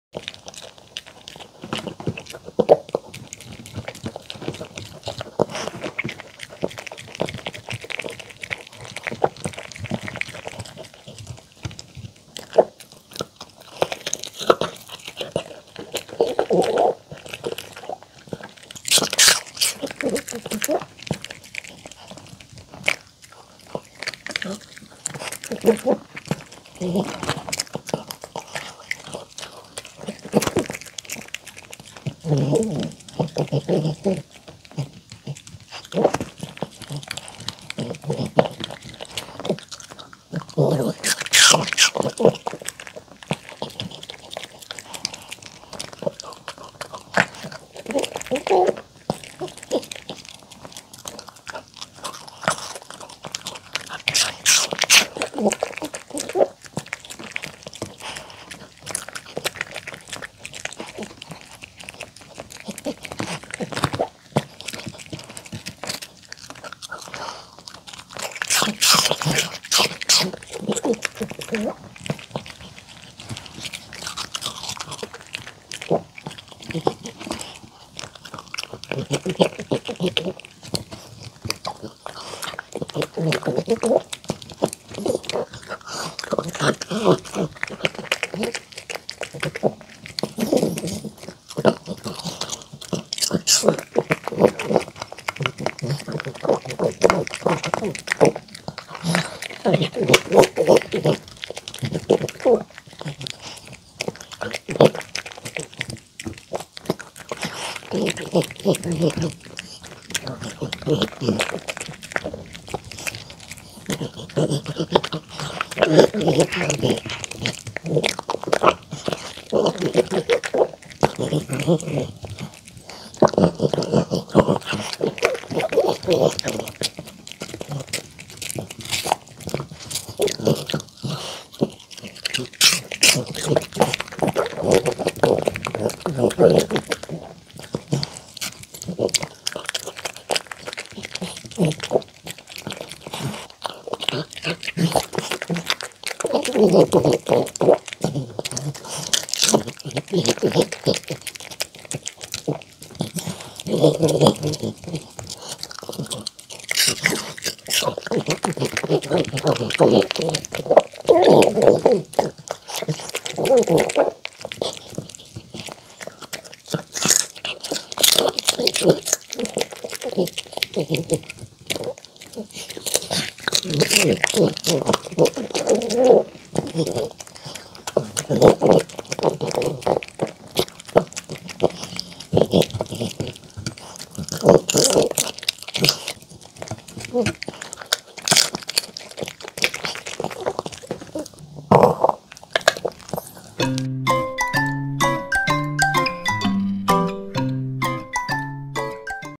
푸들 몽이 강아지 먹방 치킨텐더 샐러드 DOG ASMR MUKBANG eating show
몽이 먹방 오랫만에 찍어보았어요😁

사과,당근,양배추
직접 만든 치킨텐더에
단호박, 계란, 올리브 오일로 만든 드레싱을
뿌려주었어요🥗

사실 저희 동네가 군소음으로 인해서
전투기 또 공항 비행기 소리가 언제 날지 모르고 해서
몽이 먹방을 포기하고 있는 ..그런 상황이에요
준비 다 해놓고 소음으로 접은 적도 있었답니댜 ㅜㅜ
진짜 스트레스가.. 하하

꾸준히는 못 올리겠지만 가끔씩
몽이 먹방 올려보도록 노력할게용
기다려주셔서 감사해요🤎